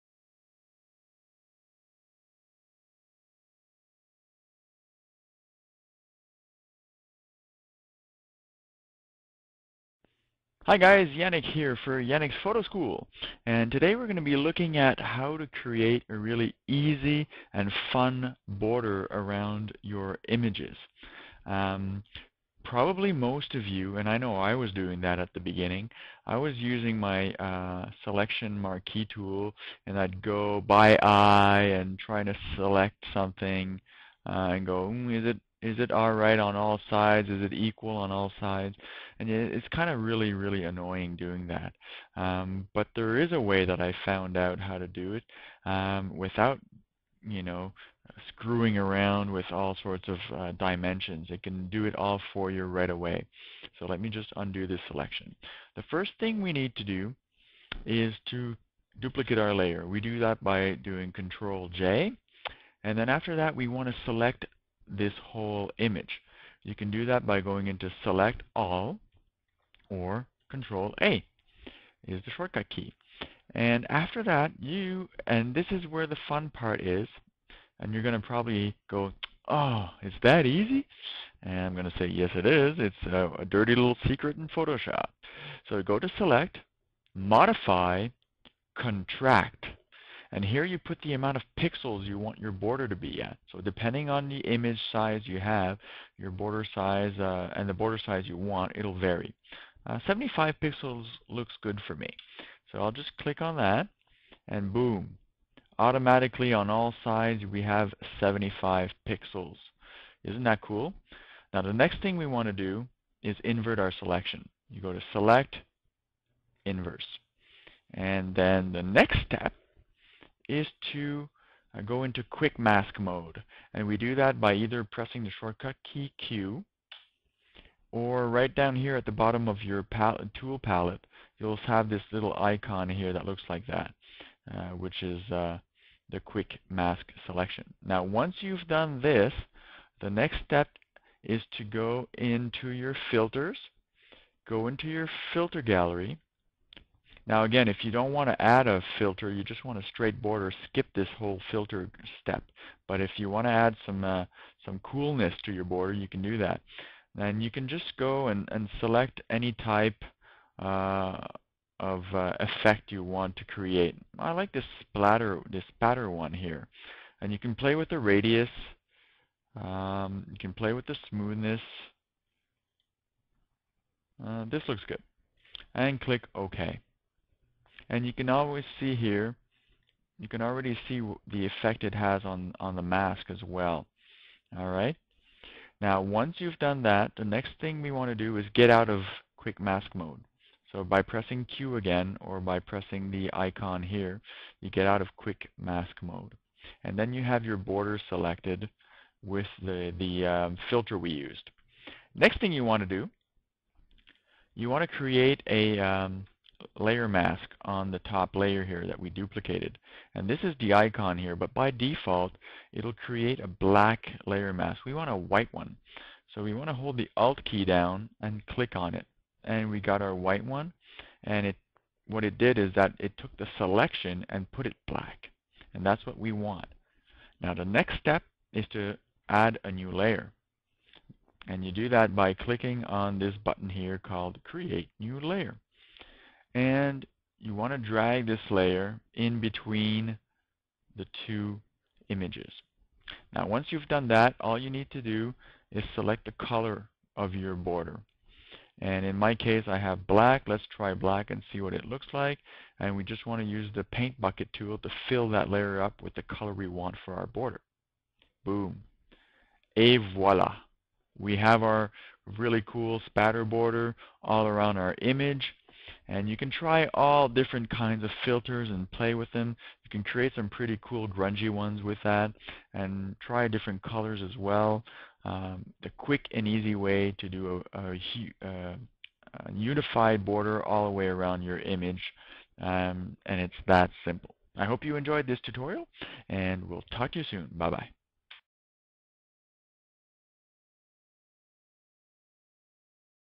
Photoshop Video Tutorial - Easy Photo Frames and Borders
Make Quick and Easy Photo Frames and Borders in Photoshop!